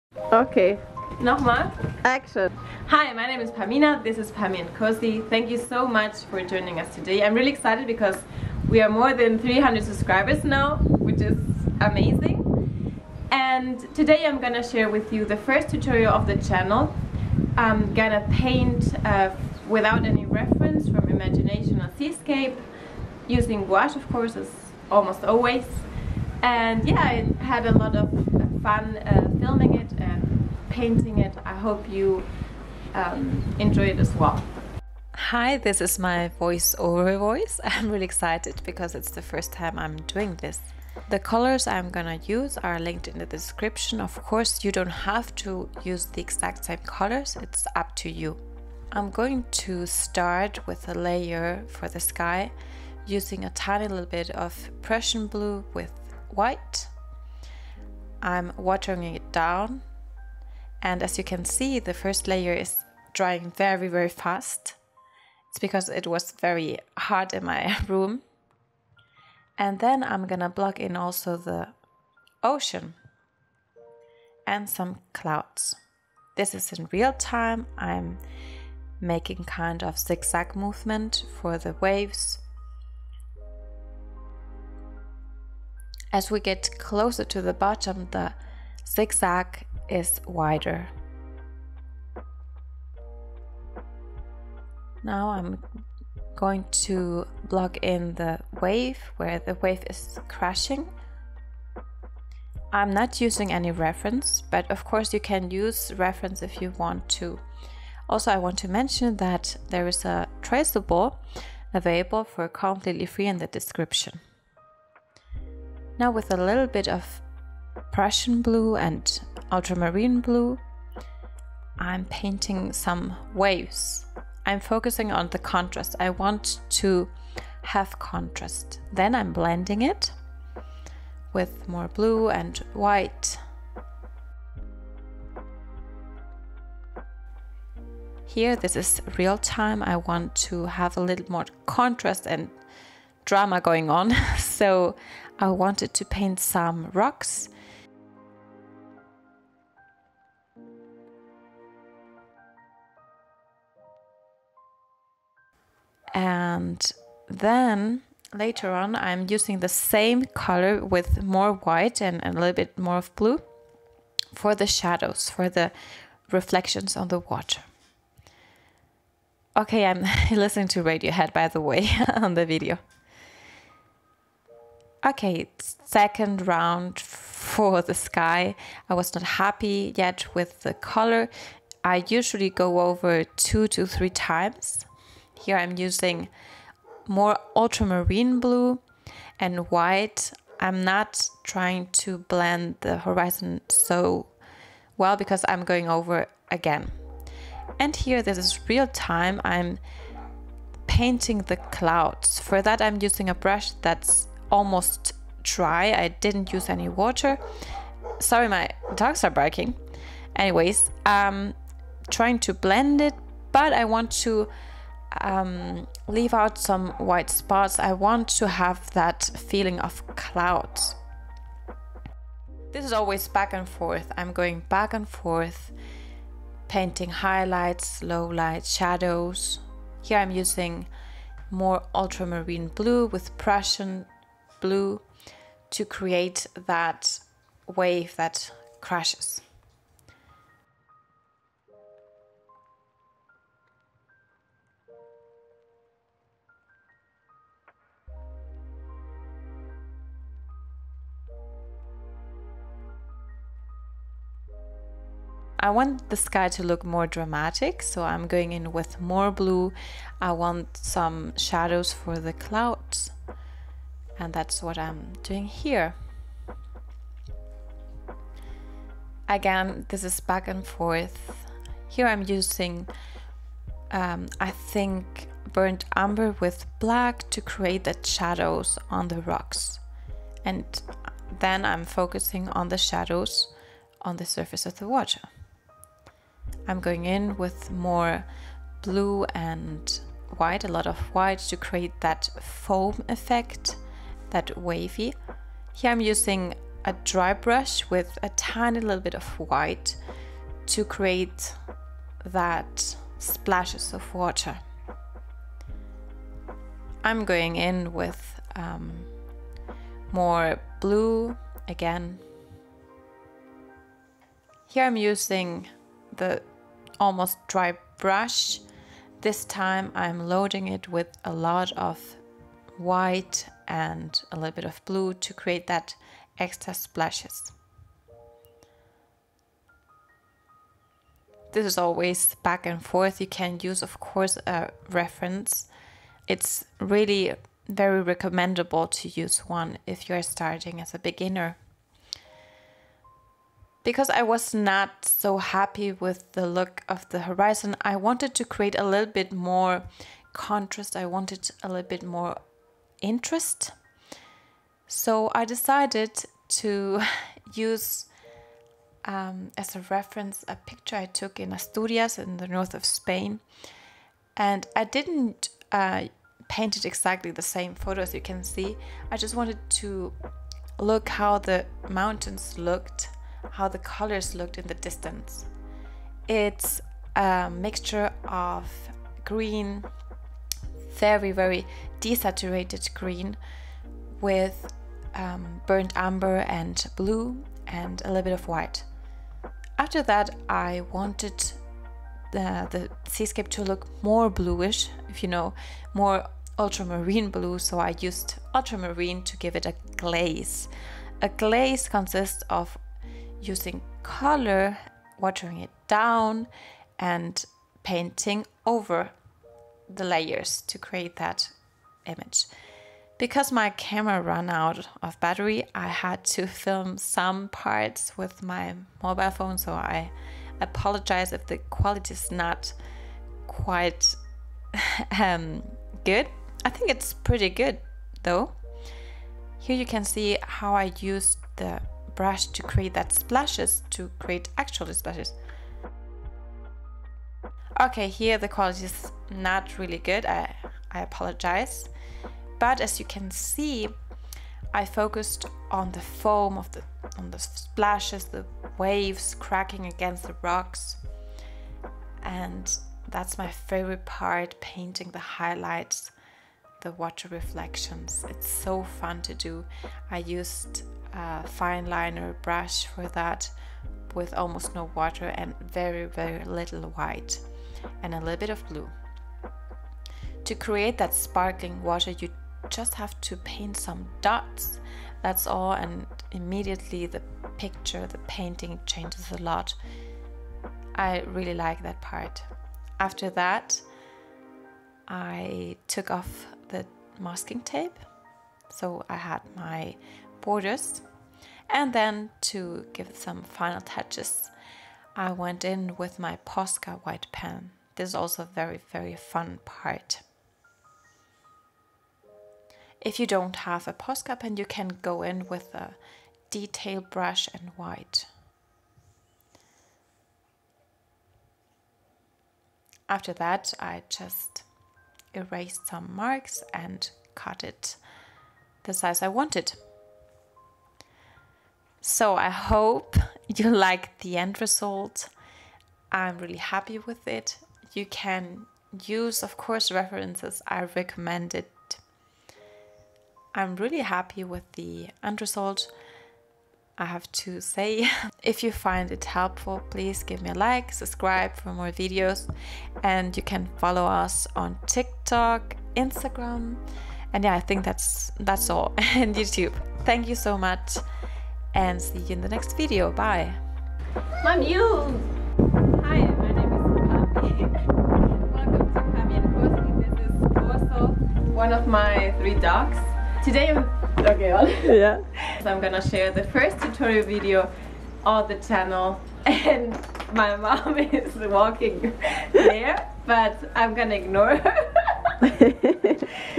🌊 How to paint a Seascape Step by Step Tutorial | ✨Painting Water and Waves + FREE Traceable
✨ This Gouache Seascape is available as a Digital Print

Hey there! It’s me, Pami 😄

↘️ To download the FREE traceable sketch just click on the and save the JPG file to your computer. As easy as that. I put a lot of effort and love into my art.

⭐ Materials used:
- Canson XL Aquarelle A4 format, 300 g
- Royal Talens Extra Fine Quality Gouache, Primary Set + Prussian Blue Phtalo + Burnt  Umber
- Van Gogh 194 Selected Filament Brushes, Royal Talens (n. 8, 10)
- Da Vinci Fit Detail brush size 0
- White Posca Pen size 0,7mm

⏰Timestamps:
0:00 Intro
0:48 Blocking in colors
1:17 Painting waves
2:17 Painting rocks
2:53 Painting the sky and clouds
4:44 Painting highlights and foam
5:47 Painting mountains
6:29 Blue glazing
7:17 Painting foam on rocks
7:30 Painting water reflections and highlights
8:03 Taking off the masking tape
8:12 Final touches
8:43 The end result
9:13 Outtakes and bloopers